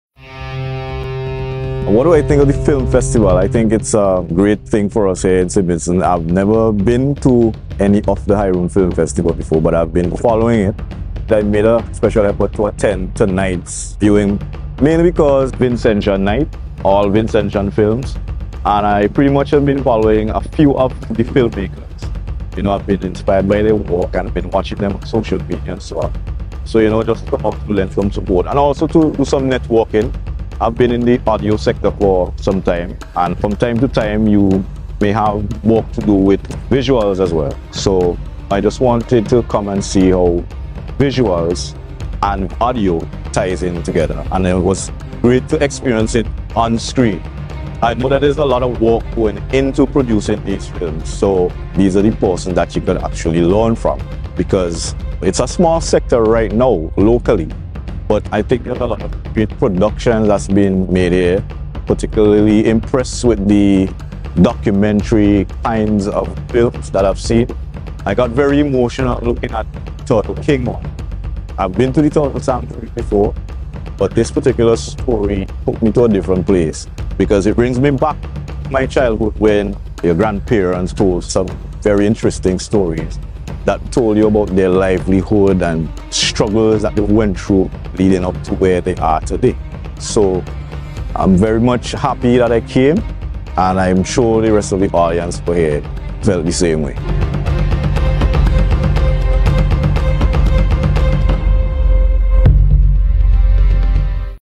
Jason Bess on Hairouna Film Festival 2023
Jason Bess shares his thoughts on the Hairouna Film Festival 2023 Vincy Film Night